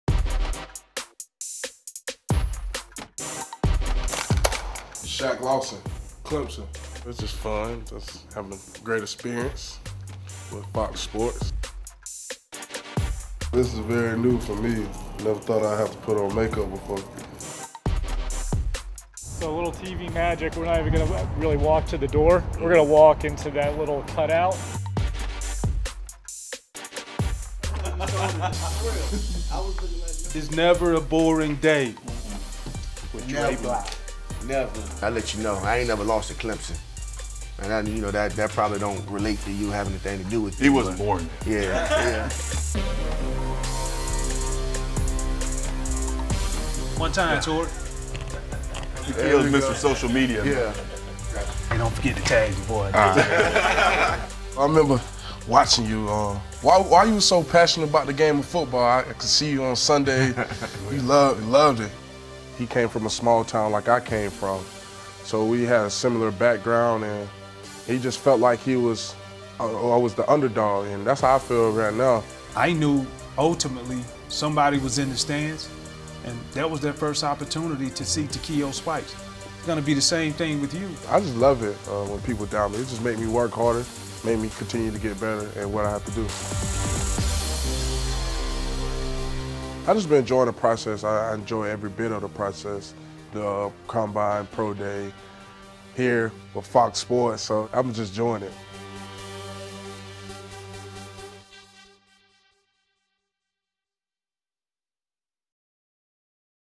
The Panel: Behind the scenes with Clemson DE Shaq Lawson
The Panel is presented by Chevrolet